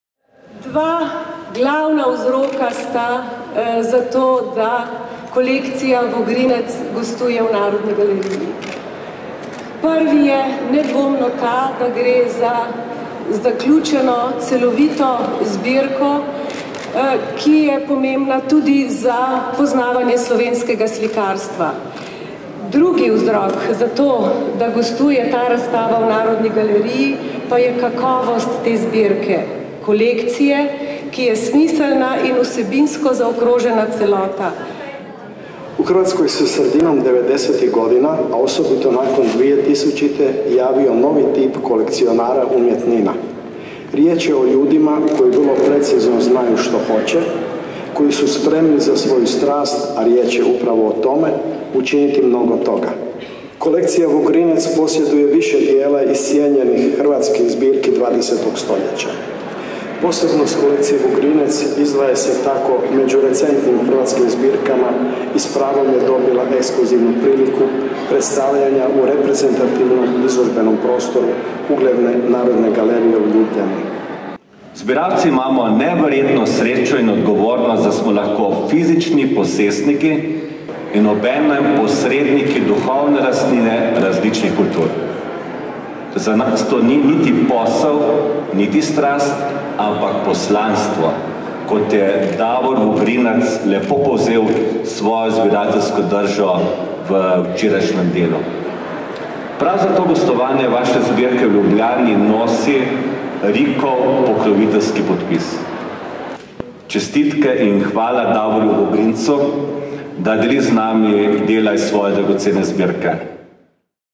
Kolekcija Vugrinec - Hrvaška moderna, odprtje razstave
10. november 2016 – 12. marec 2017
Narodna galerija, Prešernova 24, Ljubljana, Slovenija

Zbirka Davorja Vugrinca je sijajen primer zbiralske ambicije in nazorno kaže, kateri dejavniki sestavljajo uspešno zbiralno politiko. Izbor za razstavo vključuje umetnine, ki so jih ustvarili sopotniki, prijatelji in študijski kolegi slovenskih umetnikov, od Šubicev, Ažbeta, do impresionistov, še posebej zgovorno pa je predstavljeno obdobje oblikovanja "zagrebške šole" po ustanovitvi likovne akademije, ki je po sredini dvajsetih let dvajsetega stoletja postala šola slovenskim umetnikom. Na razstavo je vključenih petinšestdeset slik in del na papirju ter dvanajst kipov, dela umetnikov kot so Mato Celestin Medović, Vlaho Bukovac, Emanuel Vidović, Oton Iveković, Čikoš Sesija, Mirko Rački, Ljubo Babić, Miroslav Kraljević, Milivoj Uzelac, Marjan Trepše, Vilko Gecan, Vladimir Becić, Marino Tartaglia, Omer Mujadžić, Ivan Meštrović, Frano Kršinić idr.

Zbiratelja Davorja Vugrinca del slovenske javnosti pozna kot izjemnega športnika. Skoraj deset let je igral v turških in italijanskih elitnih klubih. V hrvaški prvi nogometni ligi je s 146 zadetki najboljši strelec vseh časov. S predstavitvijo svoje zbirke se nam razodeva njegova druga in enako uspešna plat, ki se je začela skoraj sočasno z nogometno kariero. Strast, prodorno oko, in razgledanost so usmerjale njegovo izbiro, pri nakupih pa je enako uspešen strelec kot na zelenici – vedno umirjen, s spoštovanjem do soigralcev in tako tudi sam vzbujajoč spoštovanje in občudovanje.

Avtorja razstave: Petra in Davor Vugrinec
Koordinacija: Andrej Smrekar
Generalni pokrovitelj: Riko, d.o.o.
Video: Luka Hribar